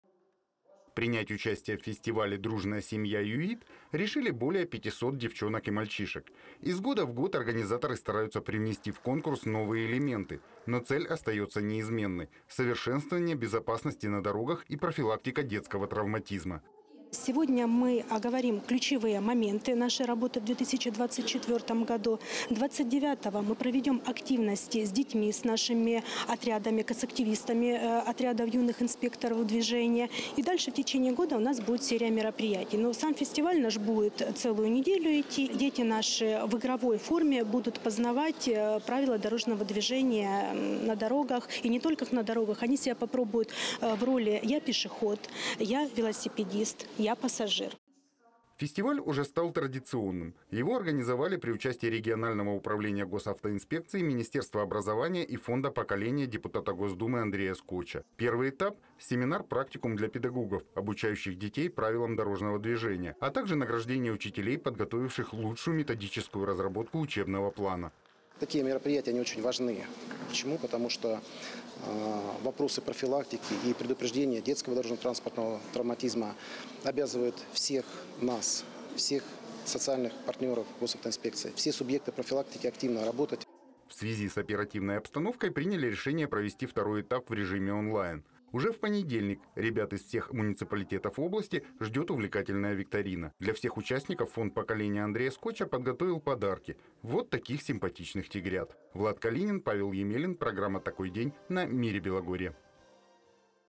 Накануне в Белгороде стартовал фестиваль «Дружная семья ЮИД»
Его цель – повысить безопасность на дорогах региона

Что рассказали детям и какие награды приготовили для призёров – рассказал Влад Калинин